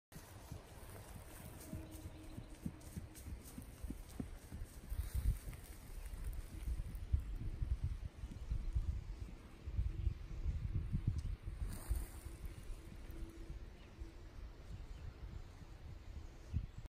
Dales Pony Kingmaker Talisman Relaxed Trot
Here's a short clip of our lovely stallion Kingmaker Talisman trotting in a relaxed way, exploring the pasture.  His trot is quite smooth to sit!